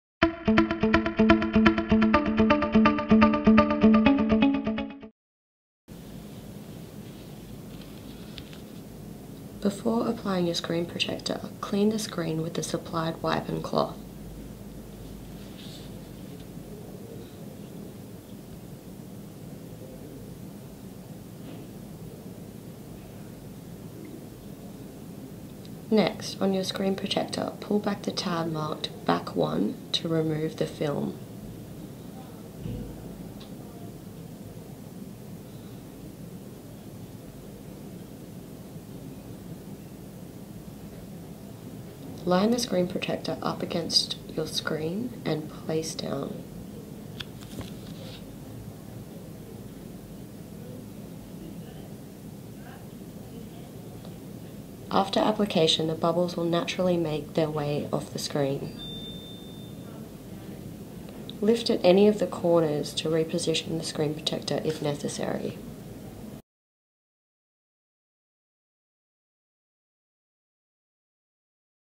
3SIXT - How to apply a glass screen protector
Protect your screen against a cracked screen. This video shows you how to perfectly apply your glass screen protector without any bubbles.